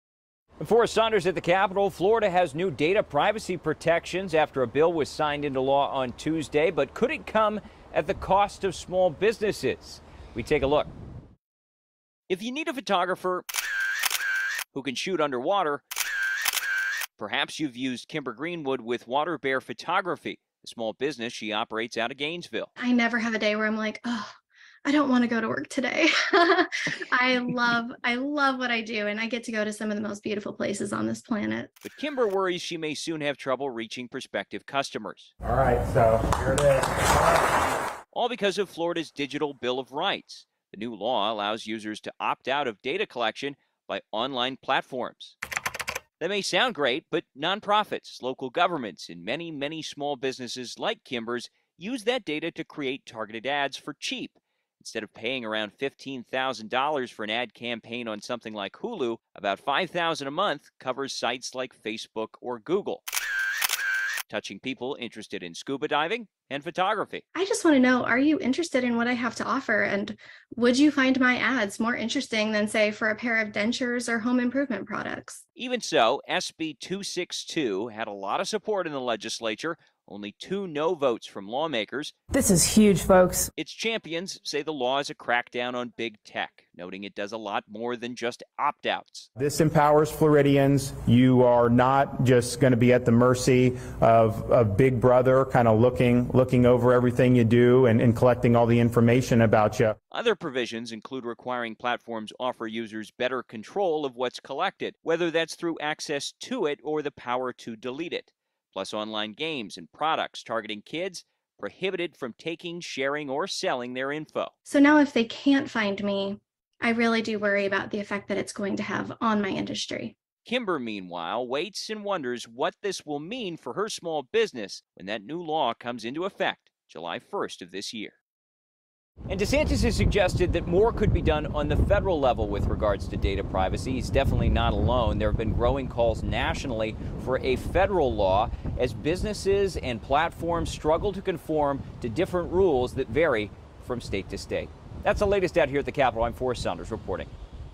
Small businesses have concerns despite DeSantis signing 'Digital Bill of Rights'
Florida’s got new data privacy protections to shield users from the collection of their online information— but it may come at a cost to small businesses. Gov. Ron DeSantis signed the “Digital Bill of Rights” in The Villages on Tuesday morning.